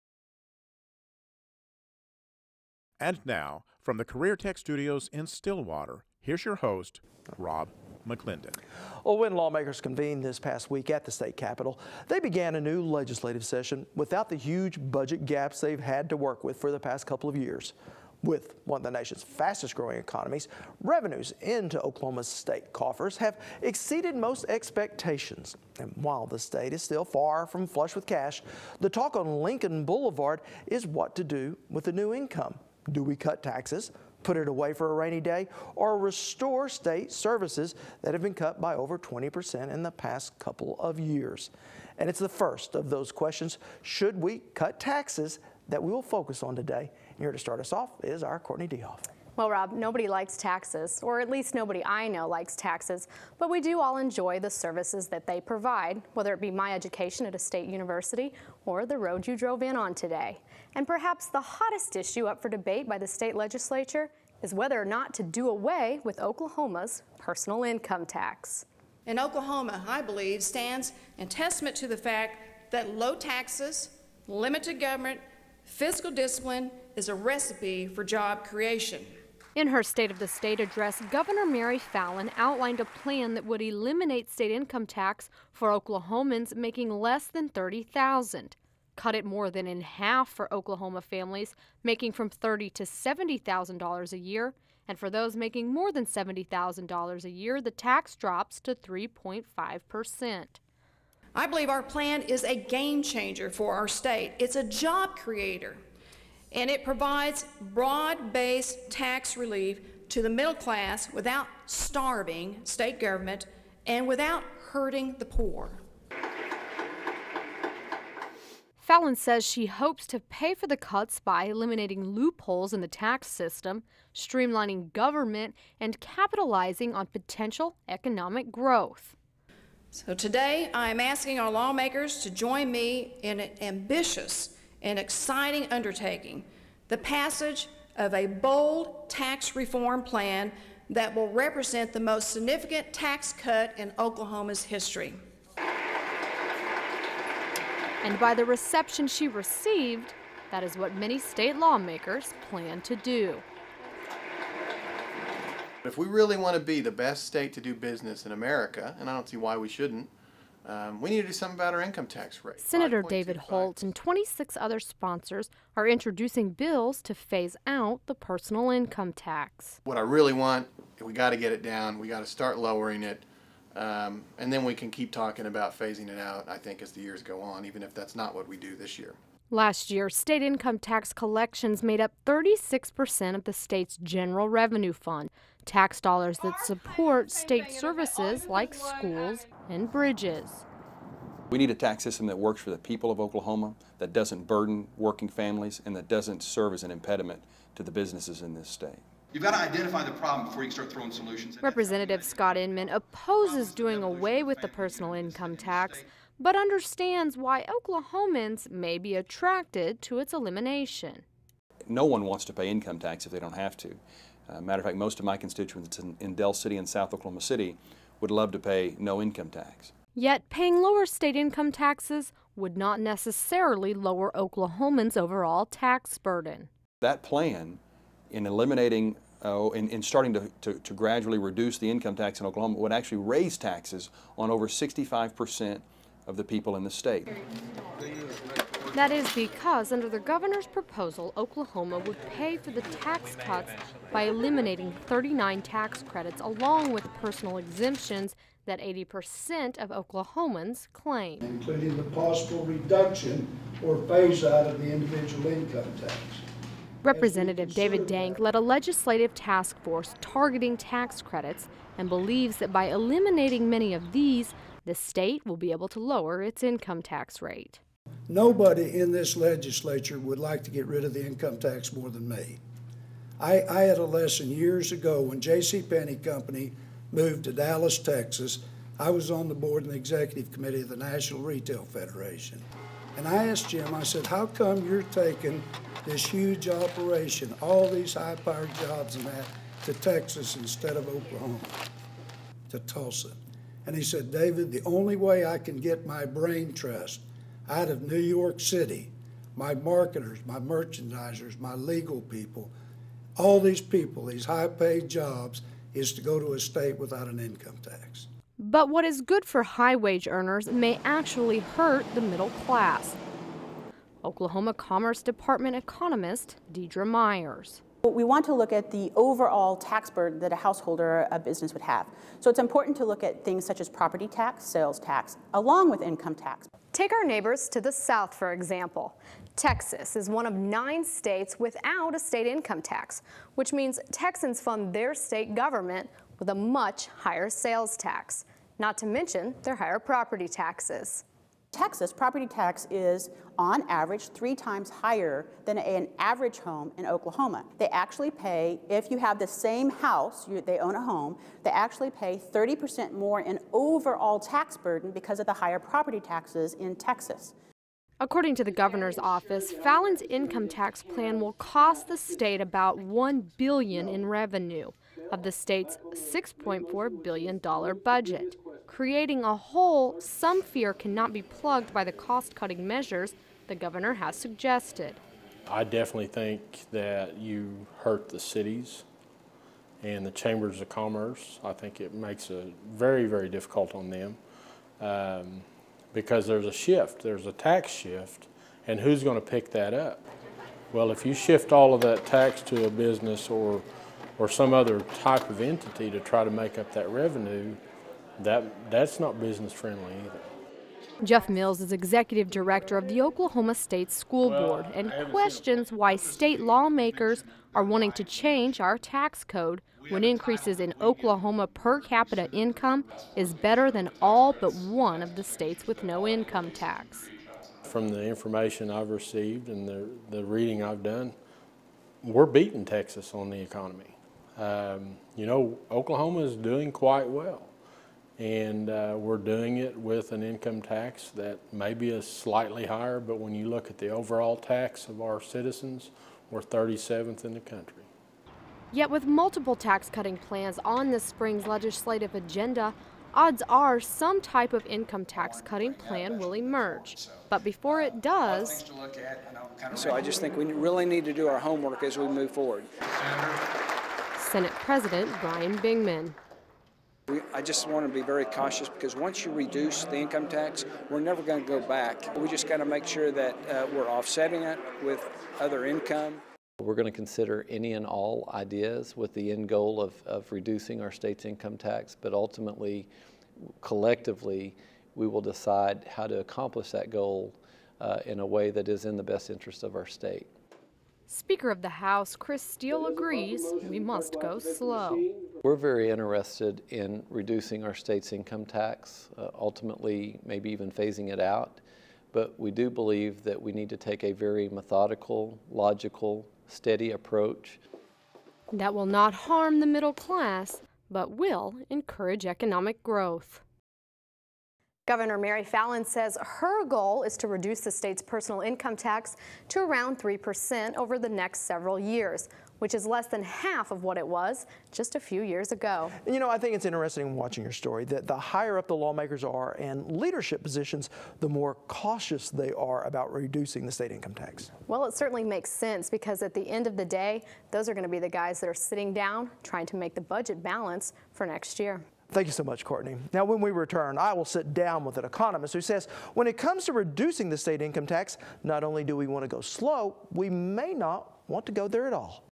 Should We or Shouldn't We? (Personal Income Tax)
We focus on the debate over whether our state taxes should be cut since state revenues are up.